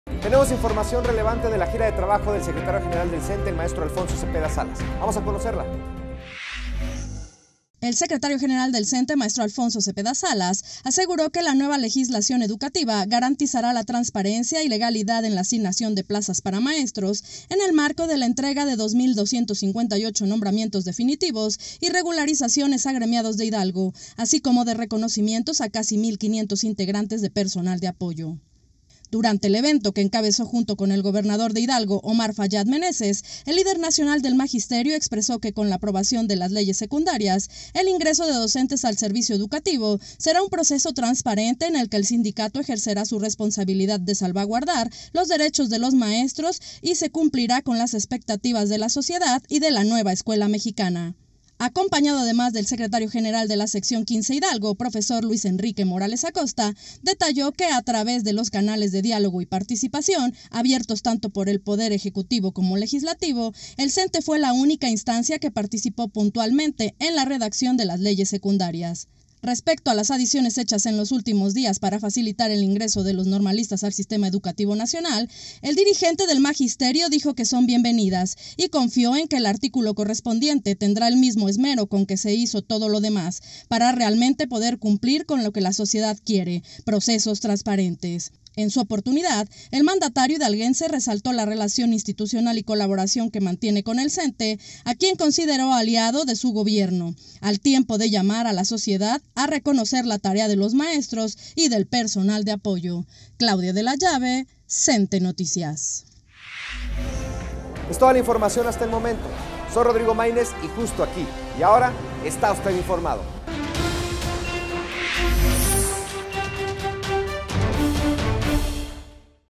Entrega de reconocimientos, como de basificación, Pachuca Hidalgo
En el marco de la entrega de 2 mil 258 nombramientos definitivos y regularizaciones a trabajadores de la educación de Hidalgo, así como de reconocimientos a casi  mil 500  integrantes de Personal de Apoyo y Asistencia a la Educación, Cepeda Salas expresó que, con la aprobación de las leyes secundarias, el ingreso de docentes al servicio educativo será un proceso transparente, en el que el Sindicato ejercerá su responsabilidad de salvaguardar los derechos de los maestros y se cumplirá con las expectativas de la sociedad  y de la Nueva Escuela Mexicana.